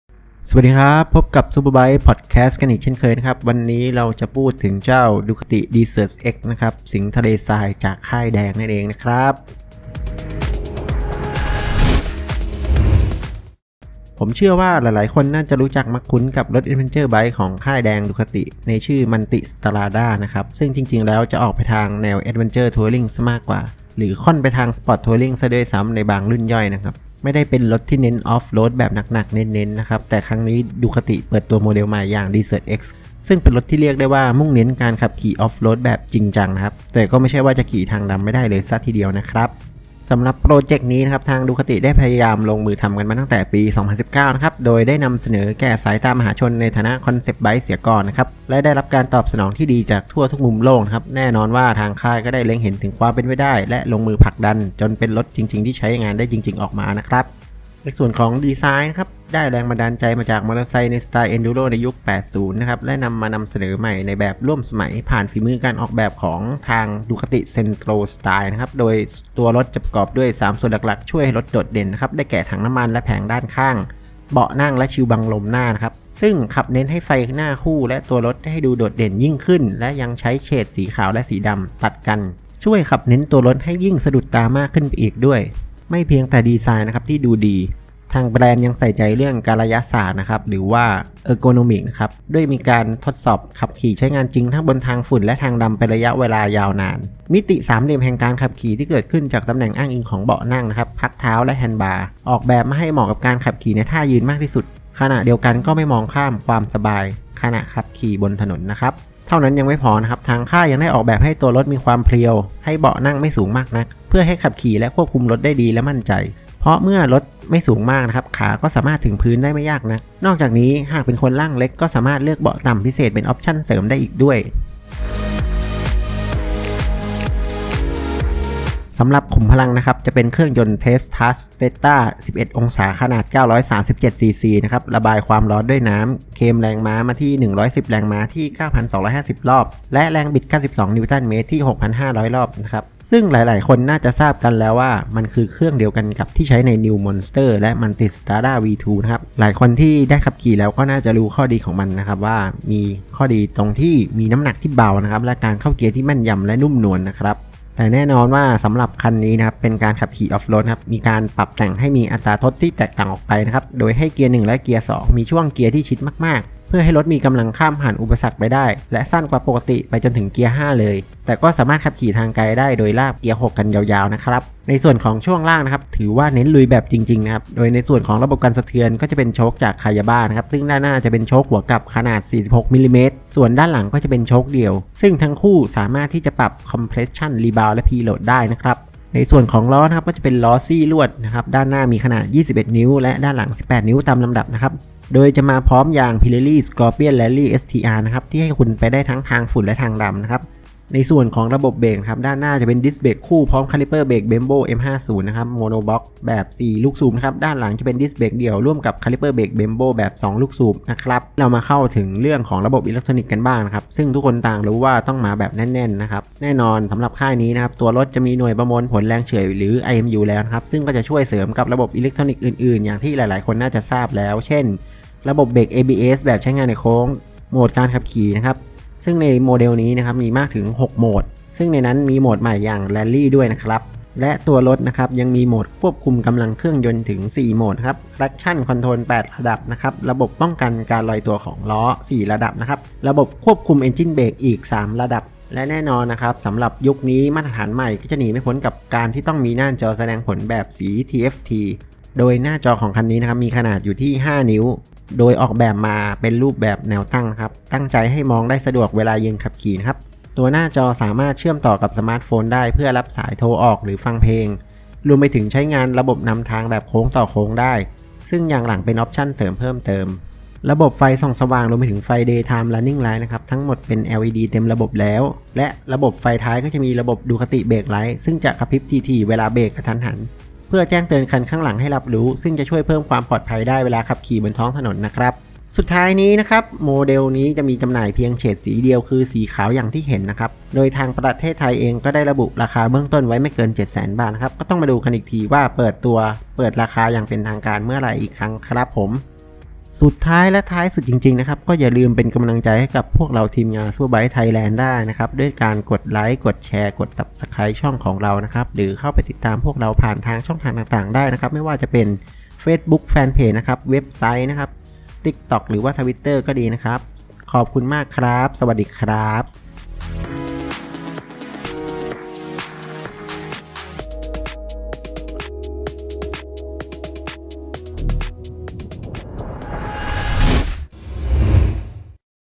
พูดถึง DUCATI DESERT X แอดเวนเจอร์ไบค์ สายลุย จากค่ายอิตาลีกันหน่อย..!!
ได้เวลาพูดถึงแอดเวนเจอร์ไบค์คันนี้เสียที Ducati Desert X สายลุยจากดูคาติ ที่ดีไซน์ออกมาได้หล่อมาก และเทคโนโลยีติดมาจากโรงงานแบบเต็มระบบ

เข้ามาอ่านลายละเอียดได้ที่ลิงก์ด้านล่าง

ร่วมเป็นกำลังใจได้ด้วยการติดตามช่องของเราคลิกลิงก์ด้านล่าง

ติดตามเราผ่านทางช่องทางต่างๆ ได้ดังนี้